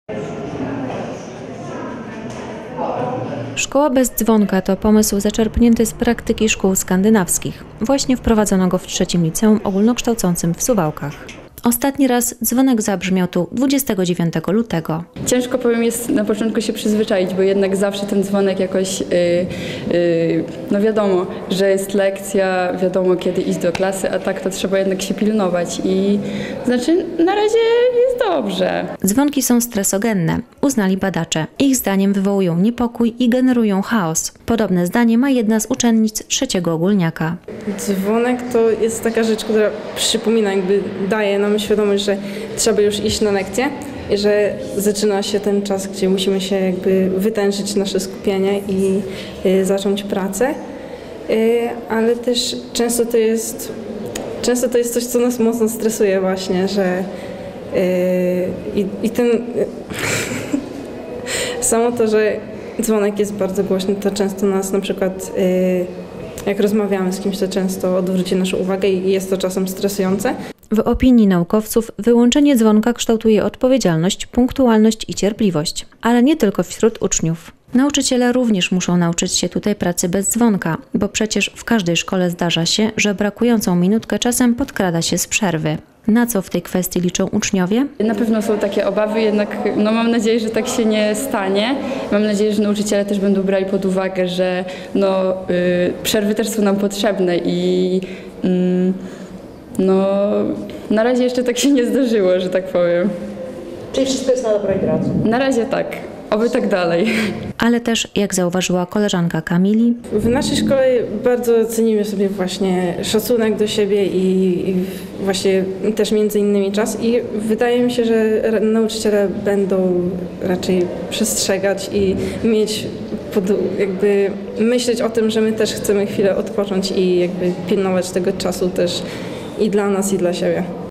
Ostatni dzwonek. |TV SUWAŁKI
Szkoła bez dzwonka to pomysł zaczerpnięty z praktyki szkół skandynawskich - właśnie wprowadzono go w III Liceum Ogólnokształcącym w Suwałkach. Ostatni raz dzwonek zabrzmiał tu 29 lutego.